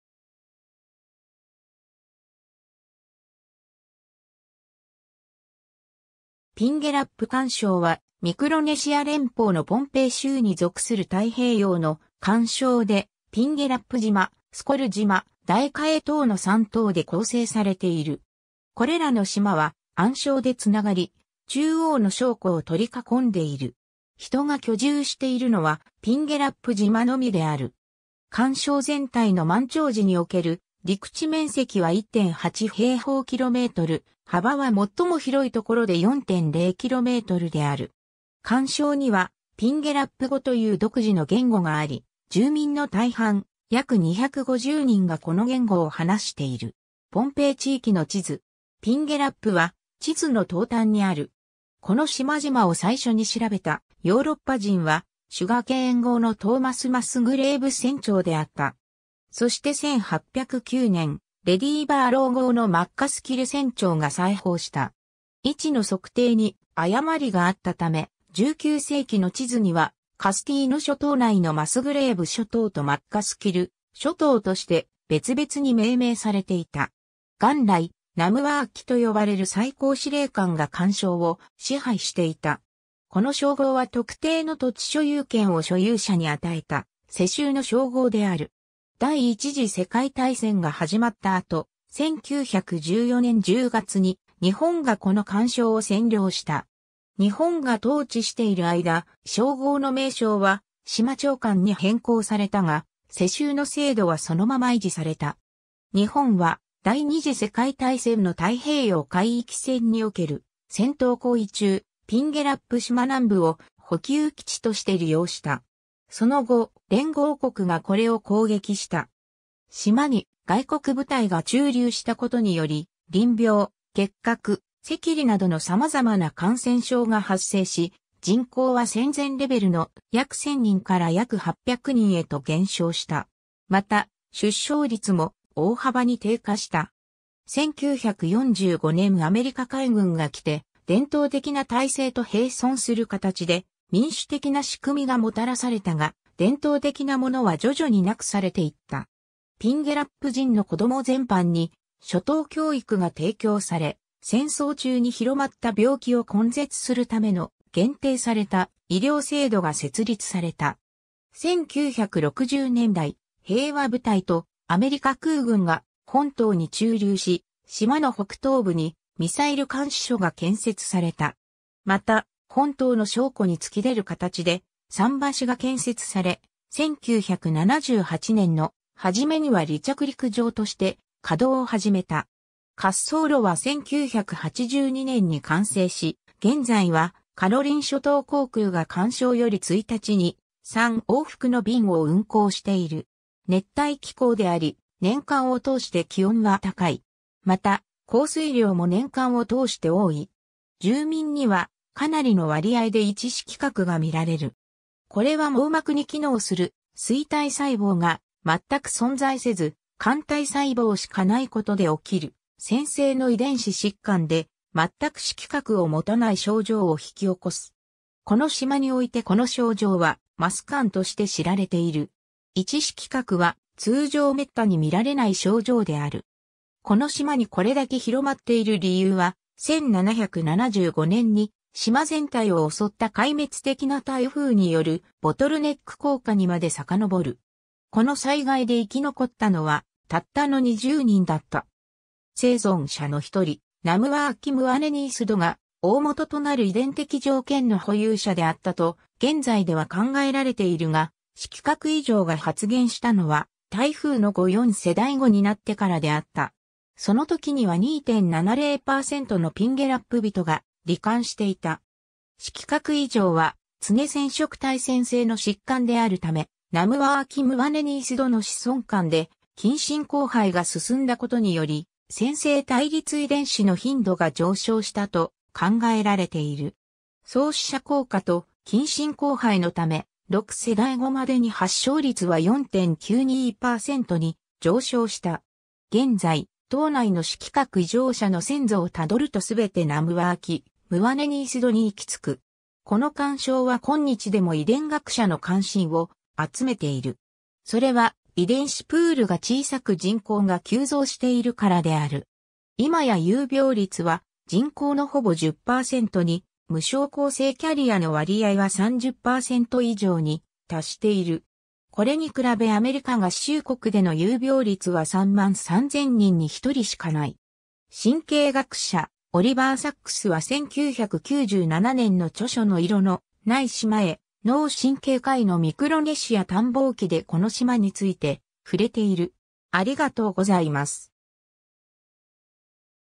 ピンゲラップ環礁
ピンゲラップ環礁（Pingelap atoll）はミクロネシア連邦のポンペイ州に属する太平洋の環礁で、ピンゲラップ（Pingelap）島、スコル（Sukoru）島、ダエカエ（Daekae）島の3島で構成されている。
これらの島は暗礁でつながり、中央の礁湖を取り囲んでいる。
人が居住しているのはピンゲラップ島のみである。
環礁全体の満潮時における陸地面積は1.8平方キロメートル (445エーカー)、幅は最も広いところで4.0キロメートル (2.5 mi)である。
環礁にはピンゲラップ語という独自の言語があり、住民の大半約250人がこの言語を話している。
 ポンペイ地域の地図。
ピンゲラップは地図の東端にある。
 この島々を最初に調べたヨーロッパ人はシュガーケーン号のトーマス・マスグレイブ（Thomas Musgrave）船長であった。
そして1809年、レディー・バーロウ号のマッカスキル（MacAskill）船長が再訪した。
位置の測定に誤りがあったため、19世紀の地図にはカスティーヌ諸島内のマスグレイブ諸島とマッカスキル諸島として別々に命名されていた。
 元来、ナムワーキ（Nanmwarki）と呼ばれる最高司令官が環礁を支配していた。
この称号は特定の土地所有権を所有者に与えた世襲の称号である。
第一次世界大戦が始まった後、1914年10月に日本がこの環礁を占領した。
日本が統治している間、称号の名称は「島長官」（Island Magistrate）に変更されたが、世襲の制度はそのまま維持された。
 日本は第二次世界大戦の太平洋海域戦における戦闘行為中、ピンゲラップ島南部を補給基地として利用した。
その後、連合国がこれを攻撃した。
島に外国部隊が駐留したことにより淋病、結核、赤痢などのさまざまな感染症が発生し、人口は戦前レベルの約1000人から約800人へと減少した。
また出生率も大幅に低下した。
 1945年アメリカ海軍が来て、伝統的な体制と併存する形で民主的なしくみがもたらされたが、伝統的なものは徐々になくされていった。
 ピンゲラップ人の子供全般に初等教育が提供され、戦争中に広まった病気を根絶するための限定された医療制度が設立された。
 1960年代、平和部隊とアメリカ空軍が本島に駐留し、島の北東部にミサイル監視所が建設された。
また本島の礁湖に突き出る形で桟橋が建設され、1978年の始めには離着陸場として稼働を始めた 。
滑走路は1982年に完成し、現在はカロリン諸島航空が環礁より1日2、3往復の便を運行している。
 熱帯気候であり、年間を通して気温は高い。
また降水量も年間を通して多い。
 住民にはかなりの割合で1色覚がみられる。
これは網膜に機能する錐体細胞が全く存在せず桿体細胞しかないことで起きる潜性の遺伝子疾患で、まったく色覚を持たない症状を引き起こす。
この島においてこの症状は「maskun」（ピンゲラップ語で「見えない」を意味する）として知られている。
 1色覚は通常滅多にみられない症状である。
この島にこれだけ広まっている理由は、1775年に島全体を襲った壊滅的な台風によるボトルネック効果にまでさかのぼる。
この災害で生き残ったのはたったの20人だった。
生存者の一人ナムワーキ・ムワネニースド（Nanmwarki Mwanenihsed、当時の統治者）が大元となる遺伝的条件の保有者であったと現在では考えられているが、色覚異常が発現したのは台風の後4世代後になってからであった。
その時には2.70%のピンゲラップ人が罹患していた。
色覚異常は常染色体潜性の疾患であるため、ナムワーキ・ムワネニースドの子孫間で近親交配が進んだことにより潜性対立遺伝子の頻度が上昇したと考えられている。
創始者効果と近親交配のため、6世代後までに発症率は4.92%に上昇した。
現在、島内の色覚異常者の先祖をたどるとすべてナムワーキ・ムワネニースドにいきつく。
 この環礁は今日でも遺伝学者の関心を集めている。
それは、遺伝子プールが小さく人口が急増しているからである。
今や有病率は人口のほぼ10%に、無症候性キャリアの割合は30%以上に達している。
これに比べアメリカ合衆国での有病率は33,000人に1人（0.003%）しかない。
神経学者オリバー・サックスは1997年の著書の色のない島へ: 脳神経科医のミクロネシア探訪記でこの島について触れている。